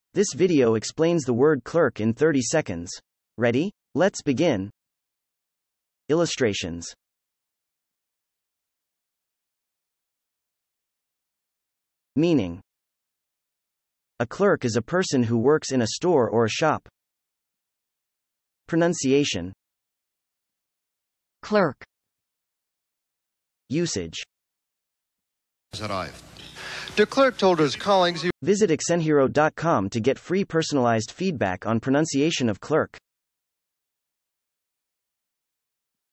CLERK - Meaning and Pronunciation
How to pronounce clerk?

This video provides examples of American English pronunciations of clerk by male and female speakers.
In addition, it explains the meaning of clerk through a definition and several visual examples.

IPA Transcription of clerk is /klˈɝk/.

Definition of clerk:
A clerk is a person who works in a store or a shop.

If you'd like to test your pronunciation of clerk, check out Accent Hero's real-time pronunciation feedback tool: